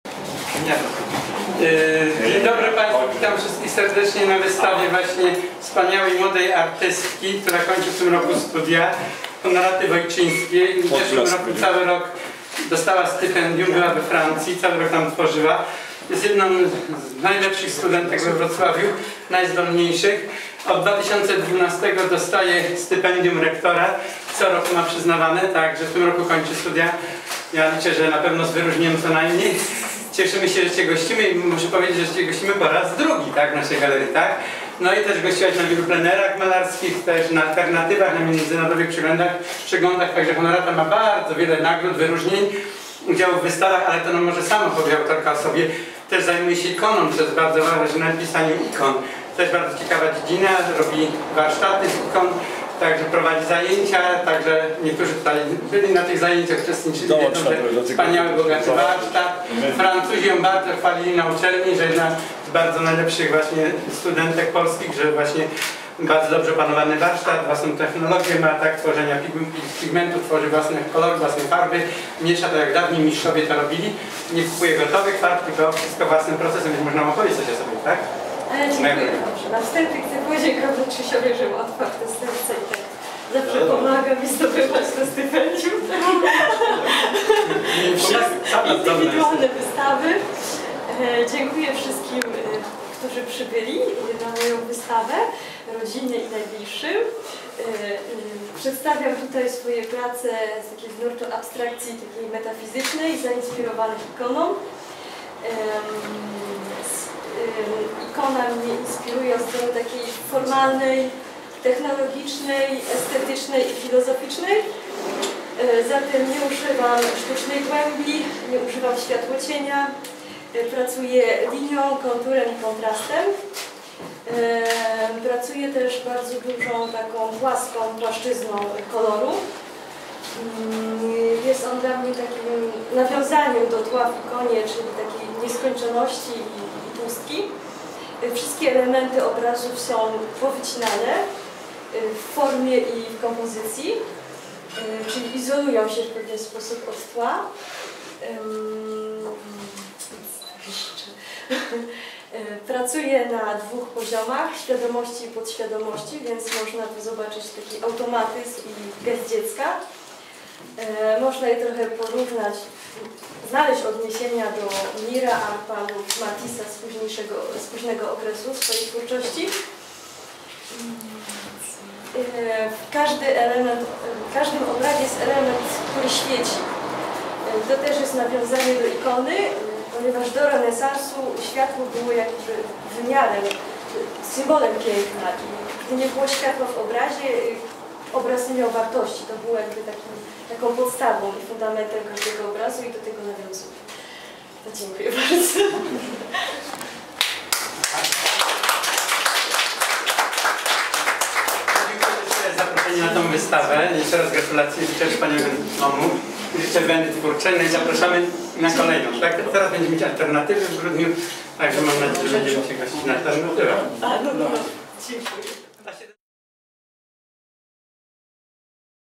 Galeria 33. Wernisaż wystawy Honoraty Wojczyńskiej
Ostrów Wlkp. W Galerii 33 swoje prace malarskie wystawiała Honorata Wojczyńska. Jej krótkie spotkanie ze sztuką zaczęło się od zajęcia pierwszego miejsca podczas egzaminu wstępnego na ASP we Wrocławiu w 2011 roku. Wystawiała w kilku miastach w Polsce oraz za granicą. Jest stypendystką Ecole superieure d"art des Pyrenees - Pau Tarbes.